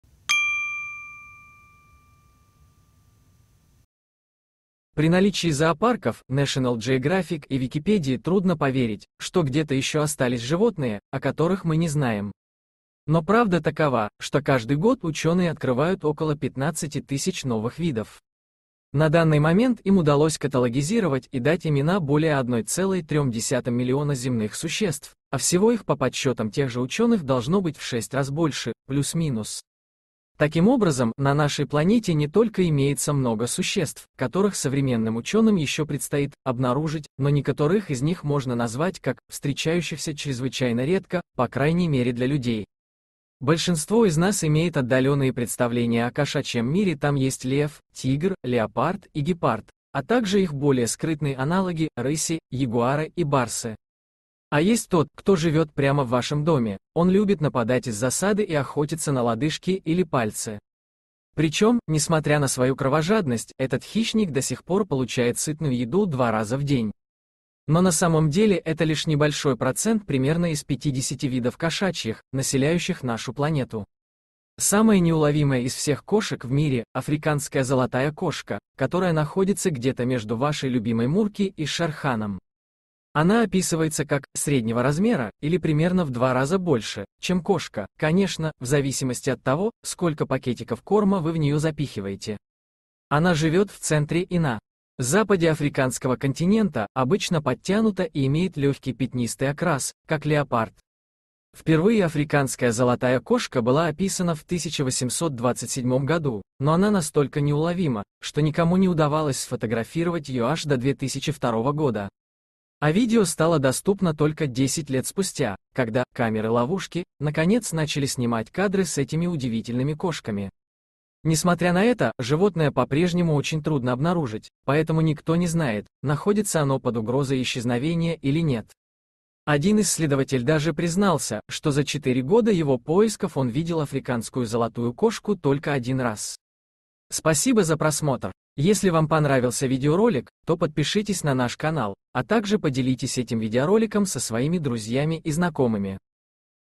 Было заснято существо с красными глазами! Шокирован весь мир!
При наличии зоопарков, National Geographic и Википедии трудно поверить, что где-то ещё остались животные, о которых мы не знаем. Но правда такова, что каждый год ученые открывают около 15 000 новых видов .